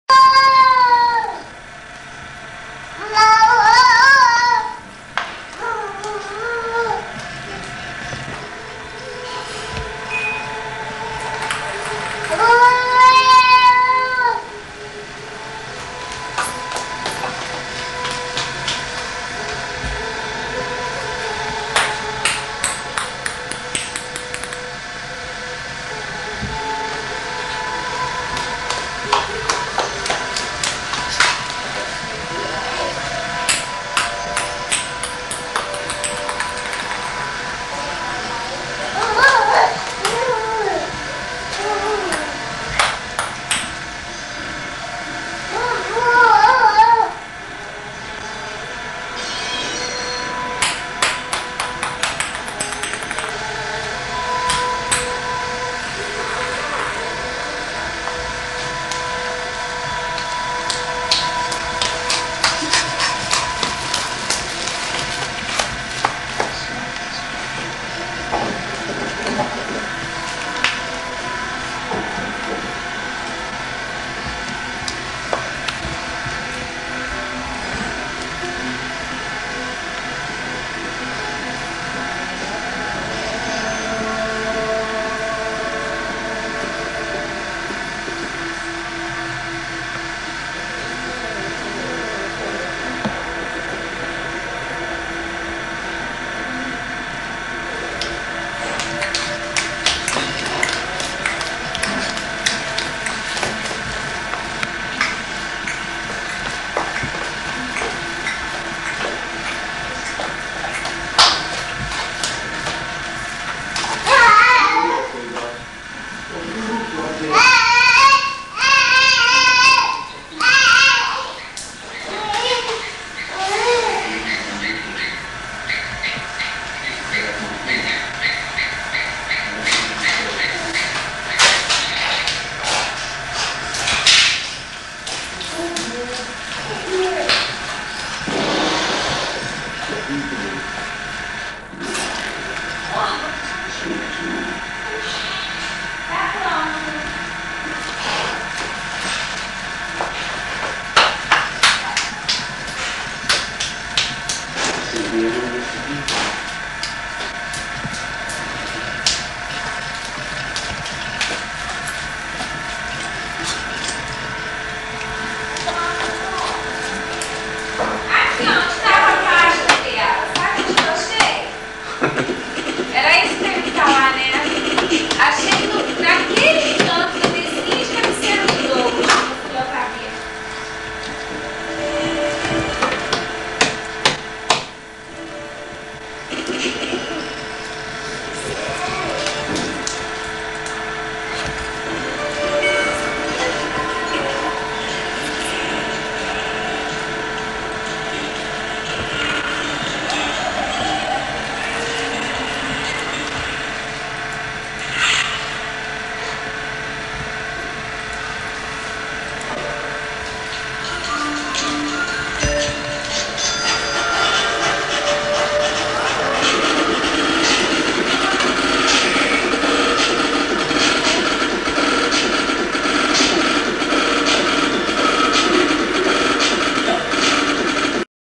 Ingrid engatinhando com Nilza - a continuação
Ingrid Bromerschenckel engatinhando e com os carinhos de Nilza